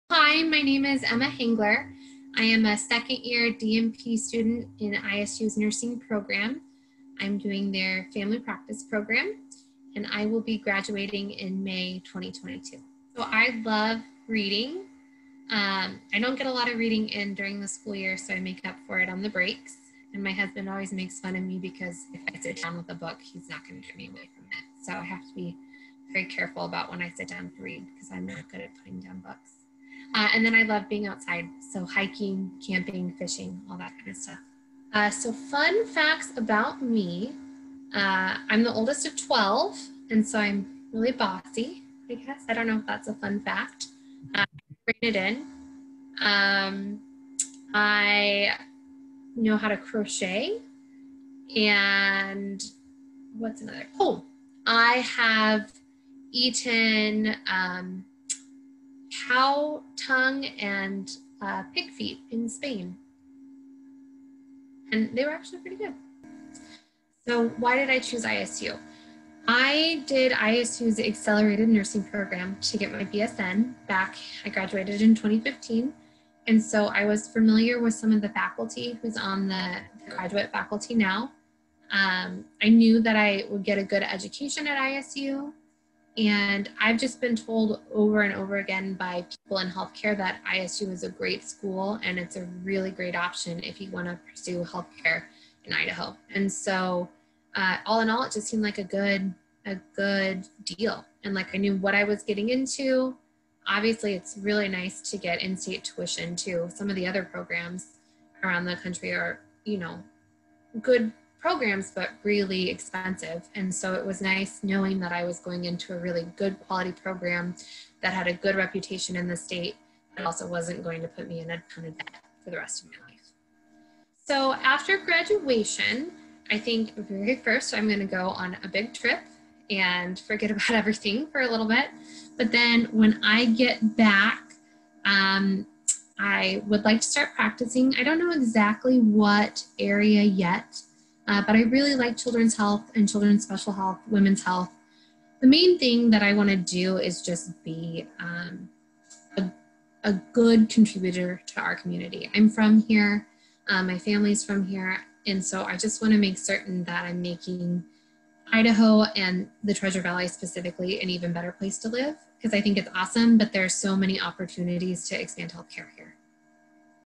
Emma Henggeler Student Spotlight
This weeks student spotlight is Emma Henggeler. Emma is a second year student in the Doctorate of Nursing Practice Program at our ISU Meridian campus.
Not only is Emma working to get her doctorate she also works part time for the Saint Lukes hospital as a registered nurse in their Neonatal Intensive Care Unit. She does all of this on top of being a good mother and wife for her family.